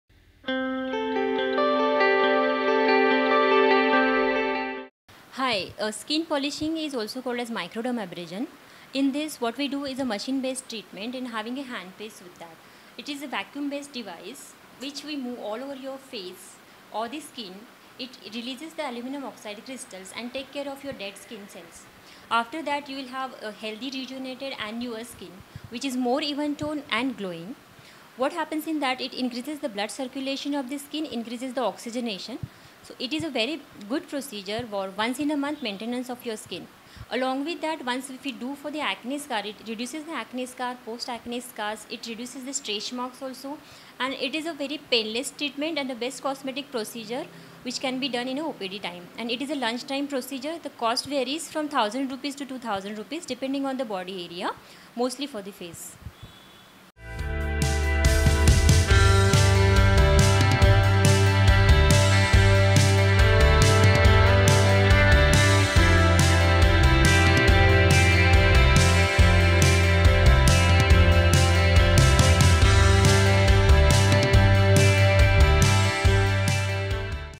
Skin Polishing (Microdermabrasion) - Treatment Procedure and Cost in India
What is microdermabrasion or skin polishing treatment? What is it used for? What does it cost in India? Dr. Sadhana Jadhavar explains about skin polishing procedure, its uses and cost of the same in India. Watch!